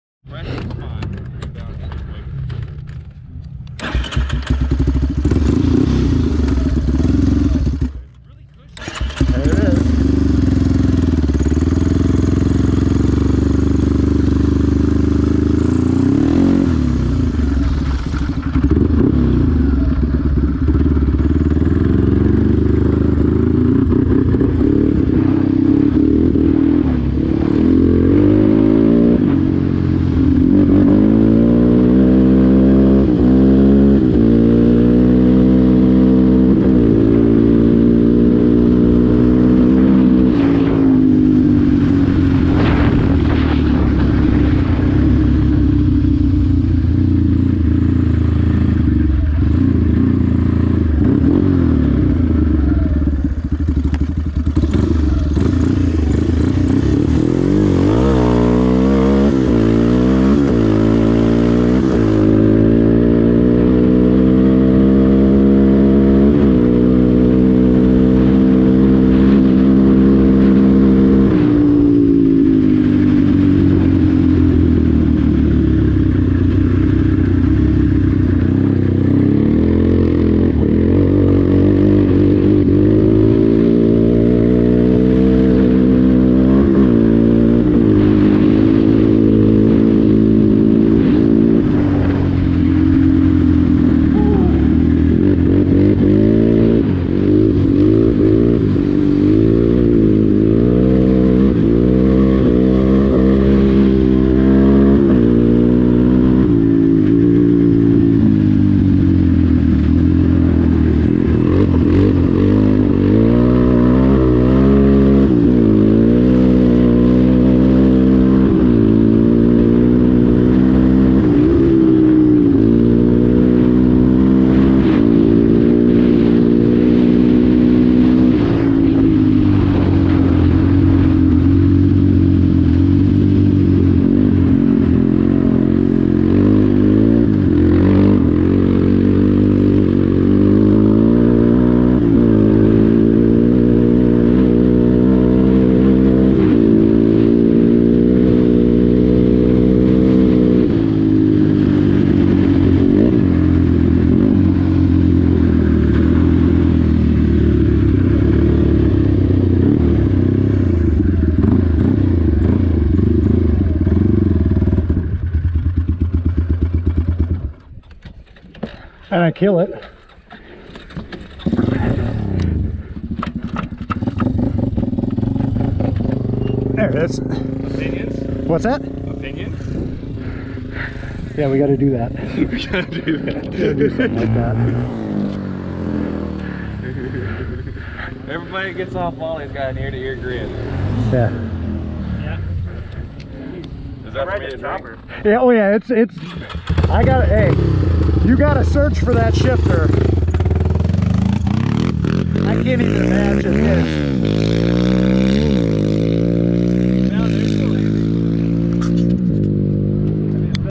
Playing with a 40+ HP Yamaha Warrior 388 @ Badlands Offroad Park
Helmet footage of me riding Weston's, owner of DRW Performance, personal machine.

There is very little still original on this machine.  it's absolutely absurd the amount this this has been customized and is a RIOT to drive.

I run all DRW products on my machine,  and after meeting Weston, I'm really glad I do.   He's got a passion for this sport,  get a hold of him if you need anything!